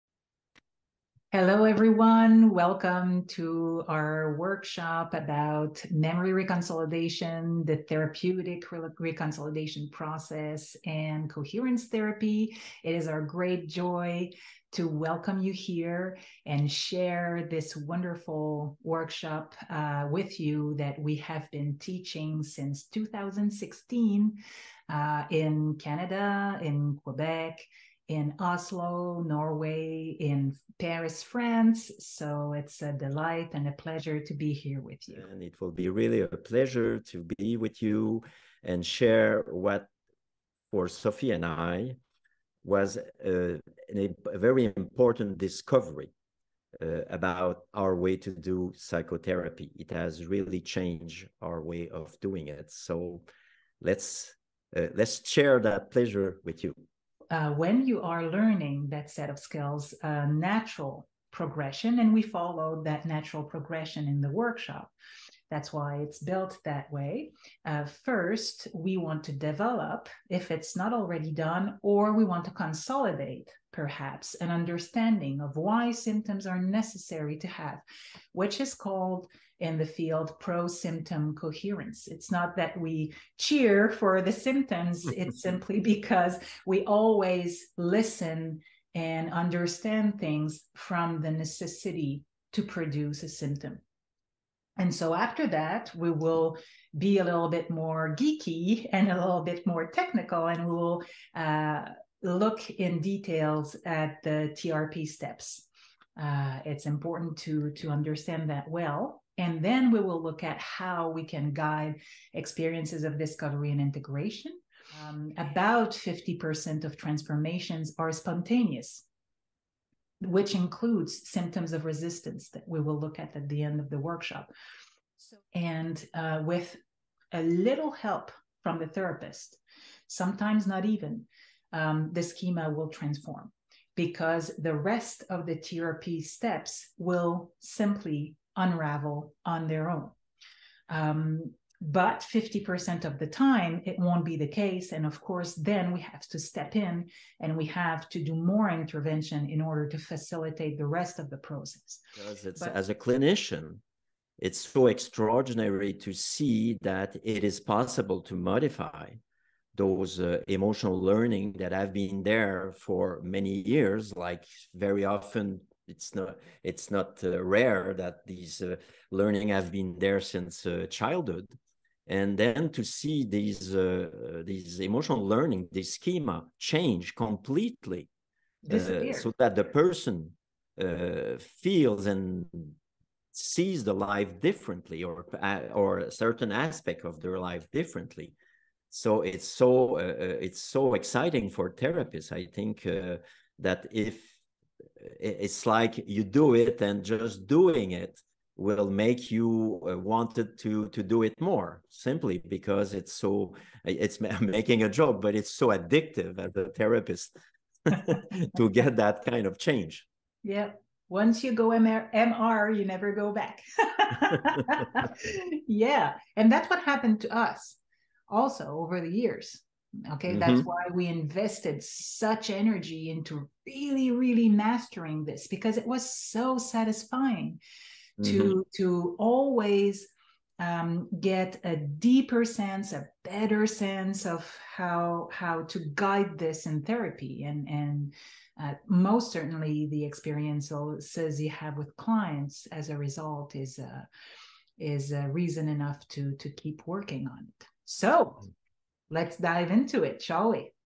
Memory Reconsolidation & Coherence Therapy course
Presentation of the training course in memory reconsolidation and Coherence Therapy, by Sophie Côté, Ph.D., psychologist with advanced certification (CPI) in Coherence Therapy and Pierre Cousineau, Ph.D., psychologist. They are both Associate Instructors at the Coherence Psychology Institute (CPI) and have co-authored two books about the Therapeutic Reconsolidation process and Coherence Therapy, as well as Schema Therapy and the Therapeutic Reconsolidation Process (Dunod, 2022 & 2023). Drs Côté & Cousineau have been teaching Memory Reconsolidation and Coherence Therapy since 2016.